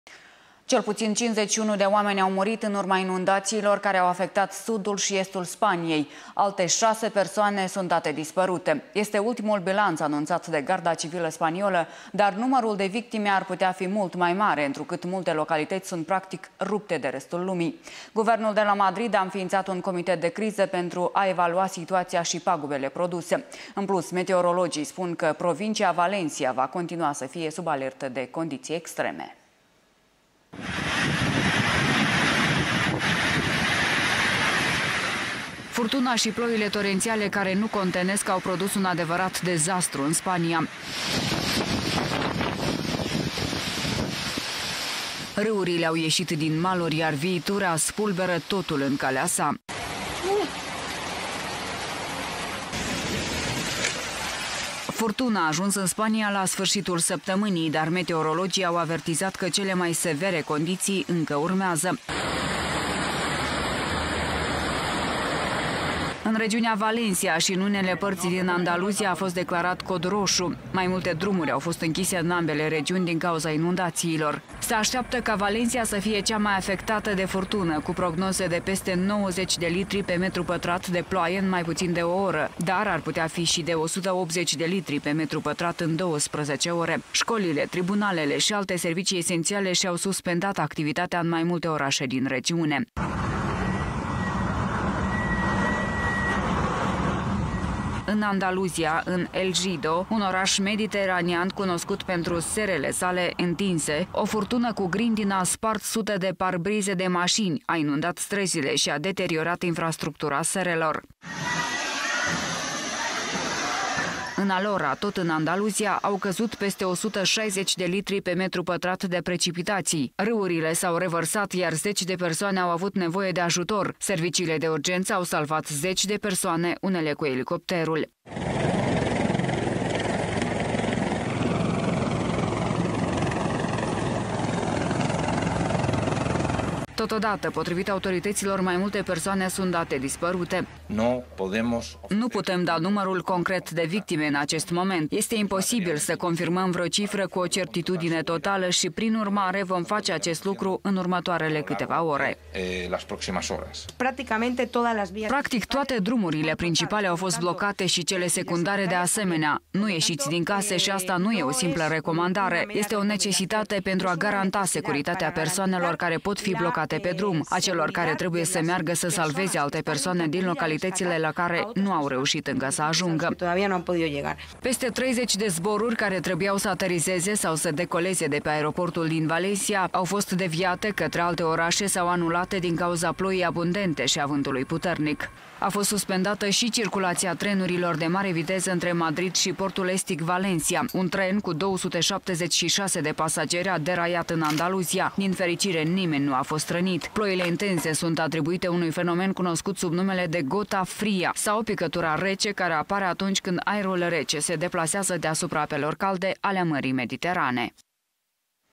Ploile fac ravagii în estul Spaniei. Cel puțin 51 de persoane au decedat
Cel puțin 51 de persoane au fost ucise în inundațiile bruște care au măturat regiunea Valencia din estul Spaniei în urma ploilor torențiale de marți, care au lăsat drumuri și orașe sub ape, au anunțat miercuri autoritățile locale, relatează Reuters.